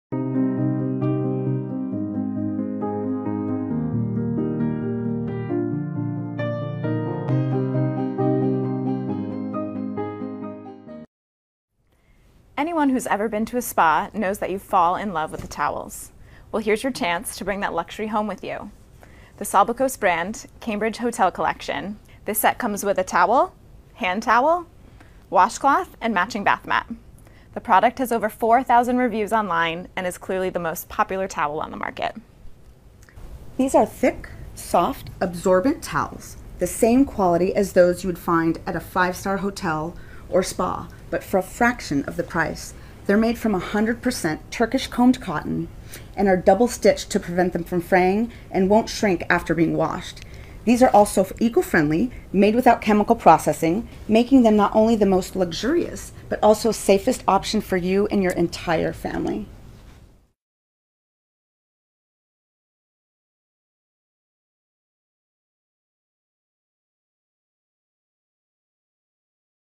🚿 Turkish Organic Cotton Towels | Amazon BEST Choice🏆
To take advantage of this amazing product go to Amazon to see the deal

🚿 SALBAKOS Turkish Towels are made in Turkey creating softer and more durable towels because it is the only country that does not use any bleach in the manufacturing process. You will feel the softness immediately and will find the perfect color to match your home décor! With Turkish Cotton, the cotton fiber opens up more and the towel actually gets softer over time.🚿